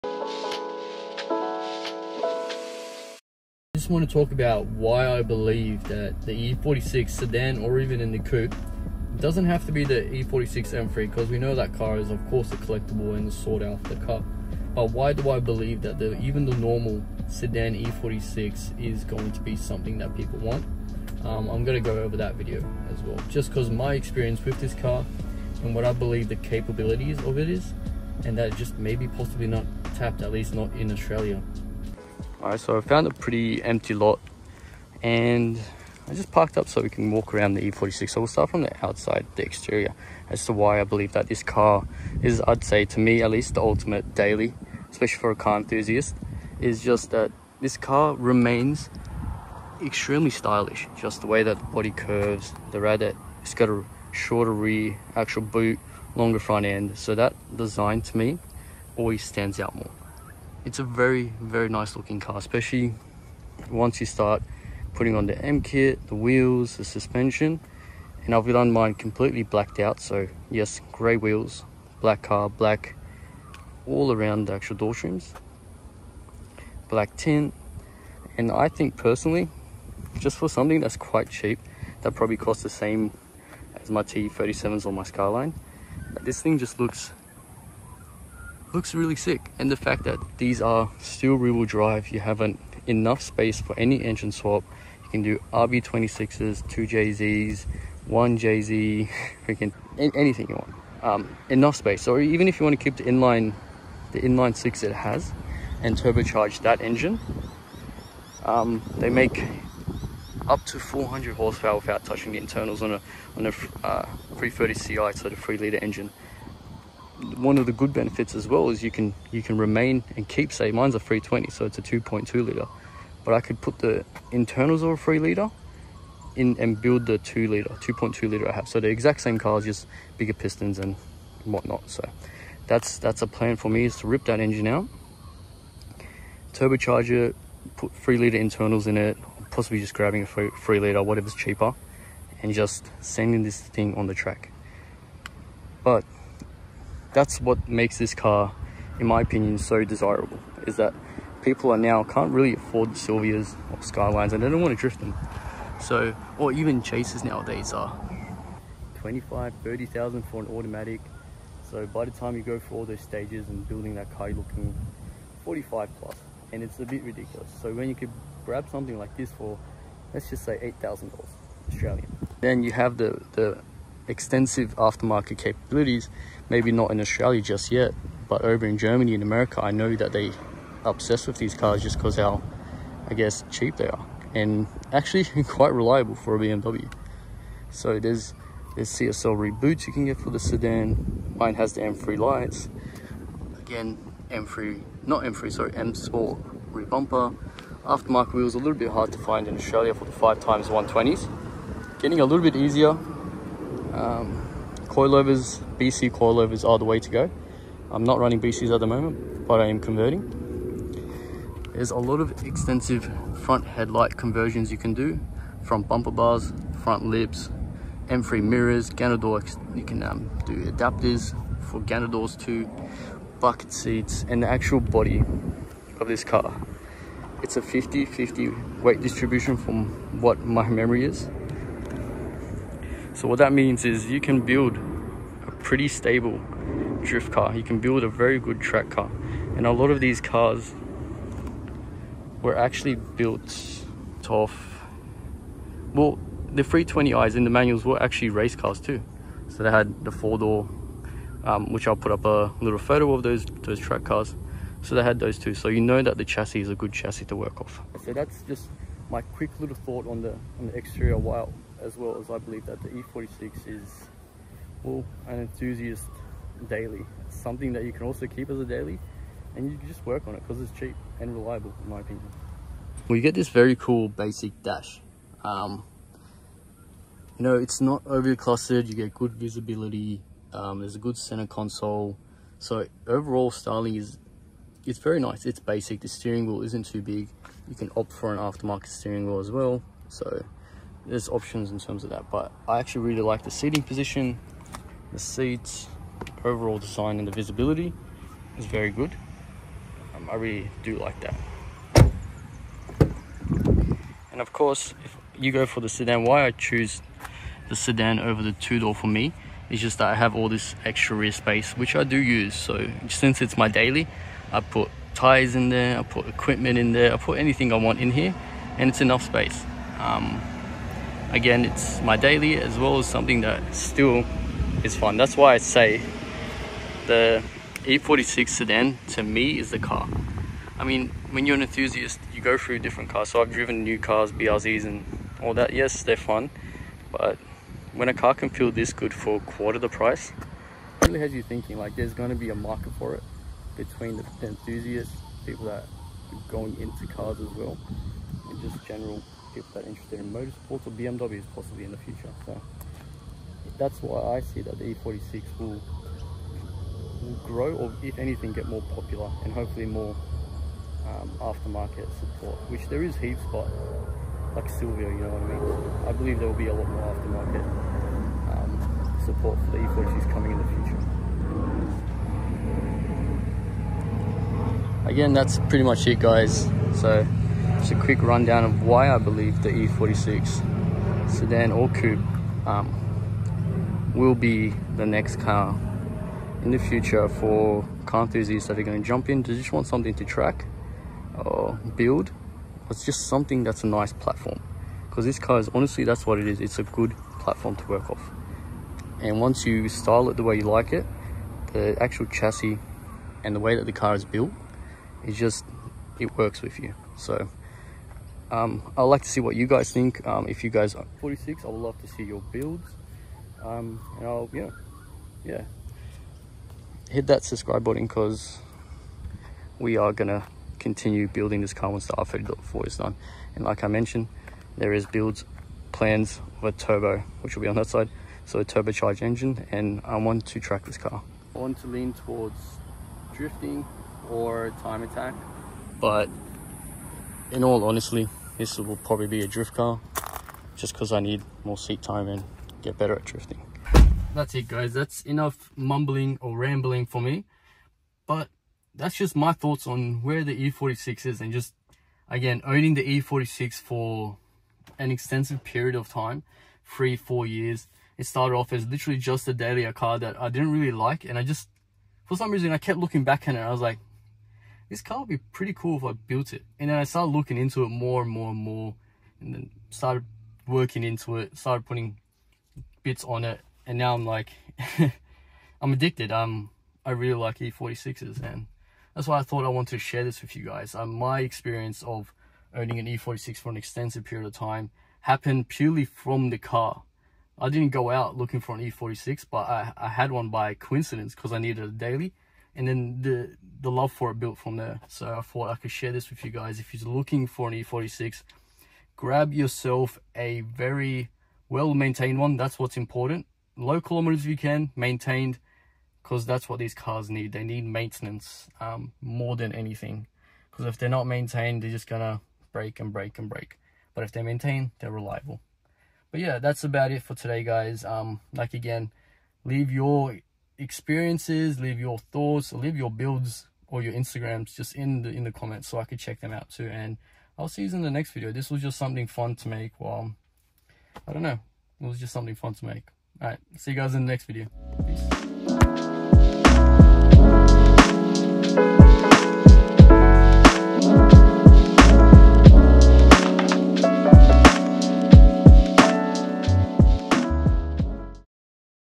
Why The BMW E46 Is The Ultimate all Around Car || Let’s Chat
Cars
E46
R34